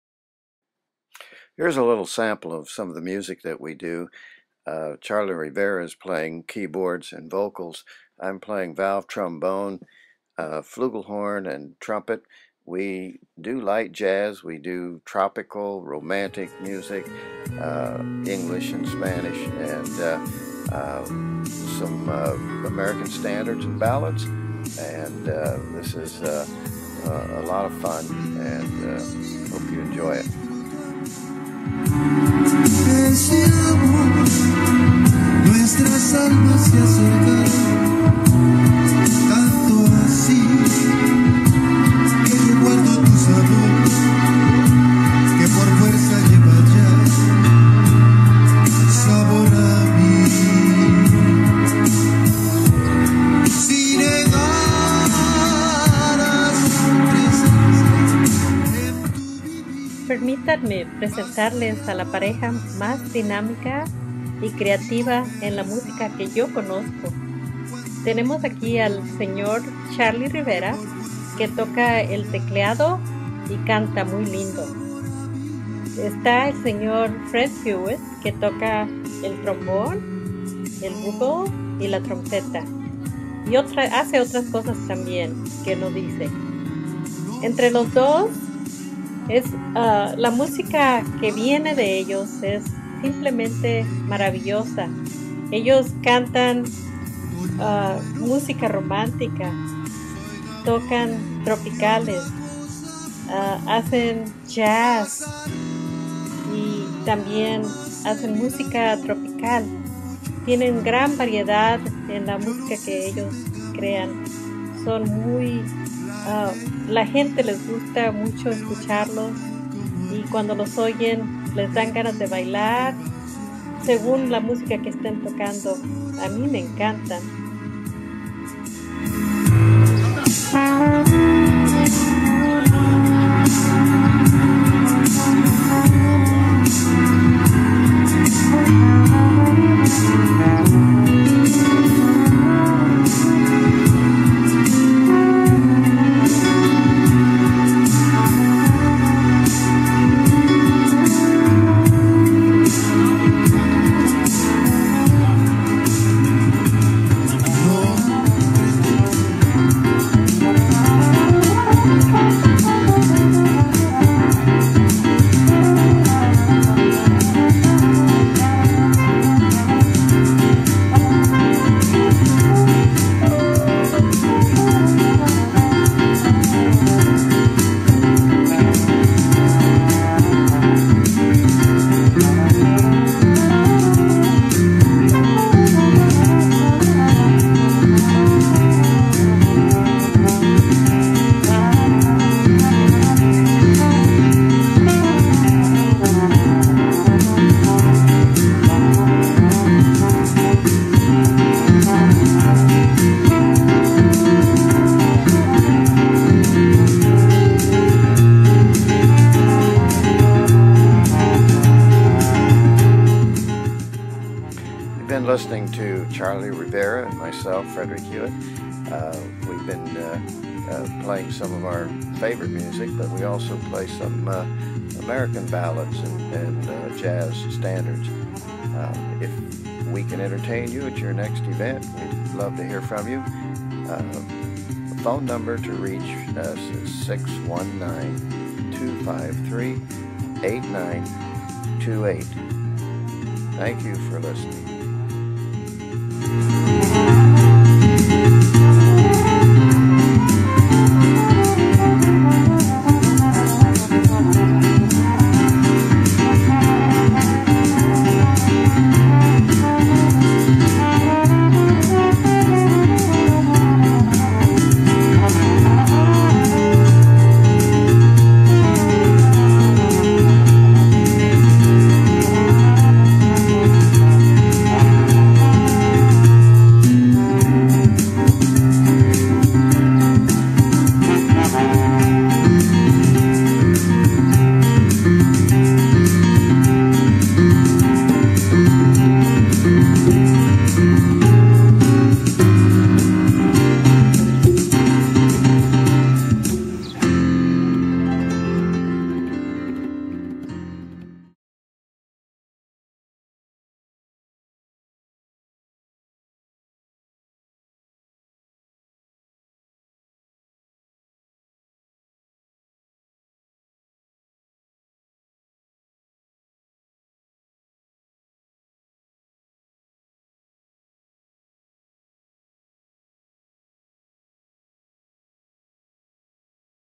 Demo Video
A quick self made video from a Yamaha S910 music workstation, directly from keyboard internal speakers to iPad movie camera.  Raw movie edited in iMovie.